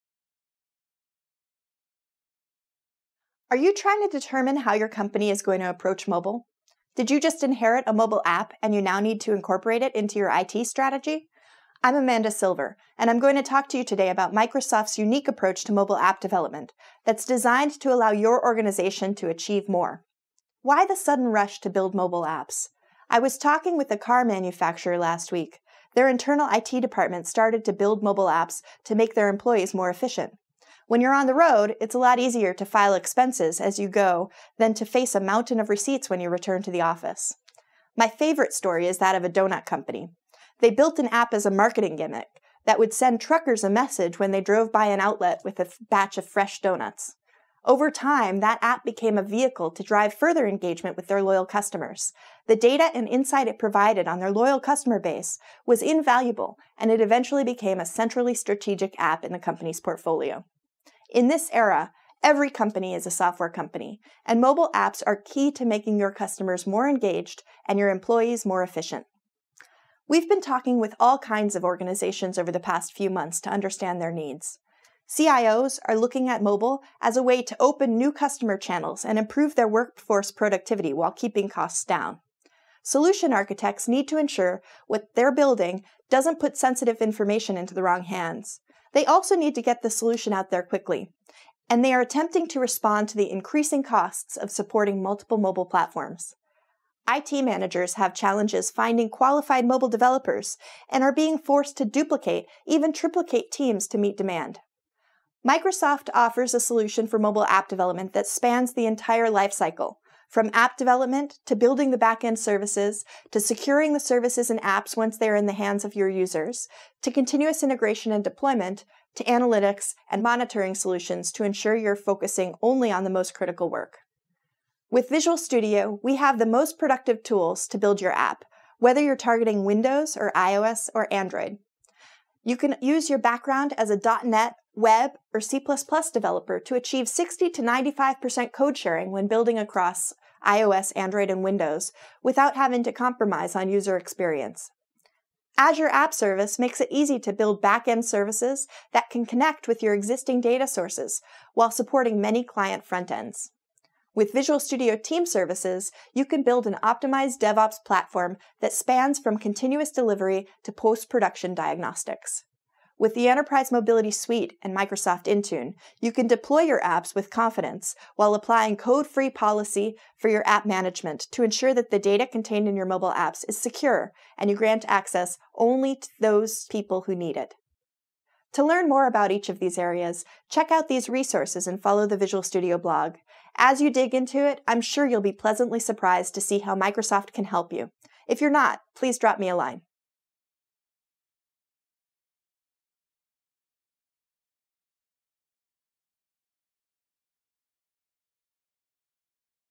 Microsoft’s Mobile Application Development Platform
We live in a mobile-first, cloud-first world where employees can achieve more and business can engage with their customers via connected experiences. Microsoft offers a comprehensive mobile development platform that enables you to deliver rich experiences to a broad set of users with reduced cost and time to market. This video takes you through the complete application lifecycle for modern mobile applications.